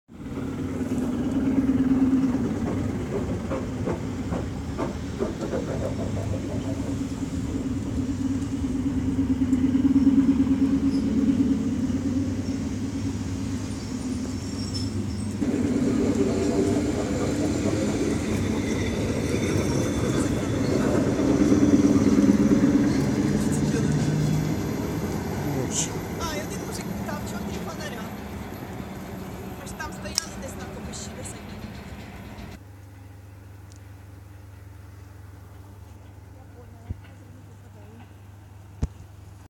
"Обгін по БОКОВОМУ" вл80т-977 и напівтемна Ер9м-541 №817 Ворожба-Фастів ст.Боярка ч2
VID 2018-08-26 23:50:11

"обгон на БОКОВОМ пути" вл80т-977 на 1 главном и по 3/7ом пути  ст. Боярка полутемная Эр9м-541 региональный поезд №817 ворожба-фастов ч2.